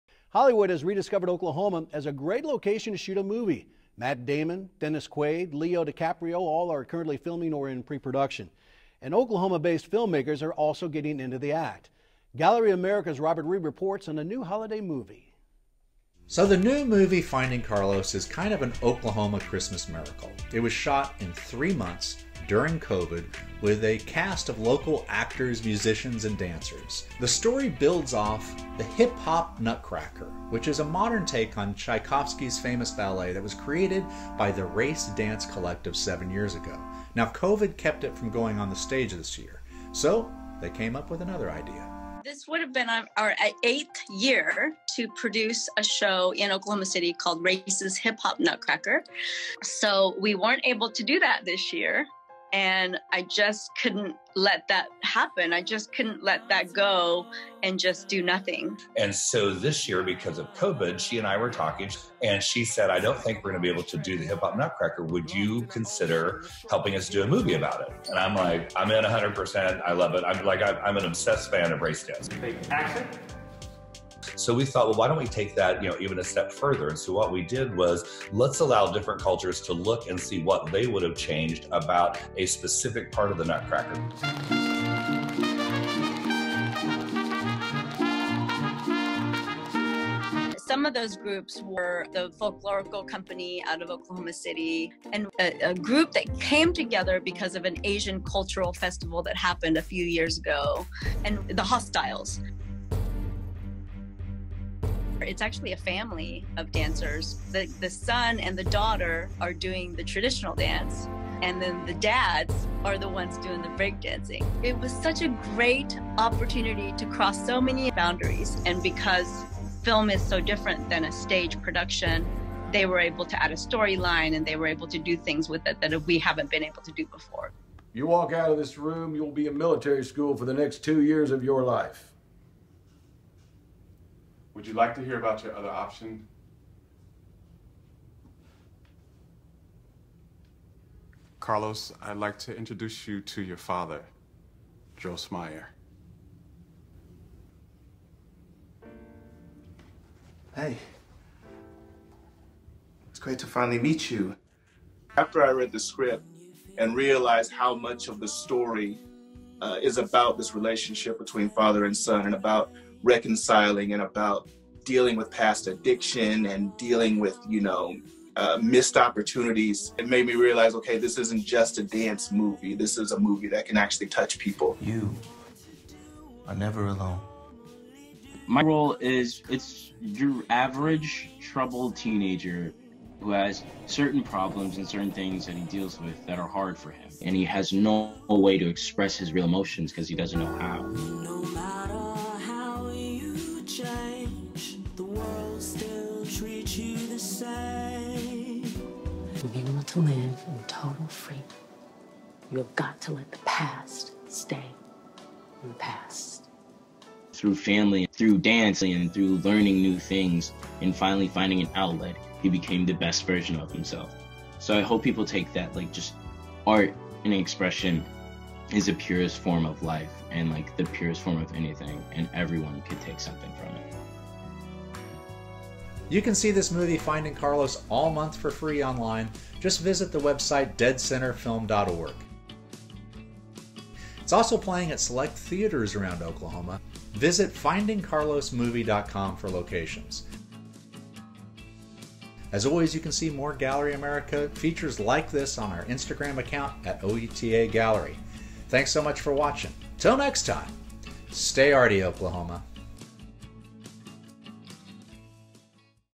Gallery America - Finding Carlos
Robert Reid shows us how the Oklahoma independent film industry is picking up with a new Christmas film, Finding Carlos.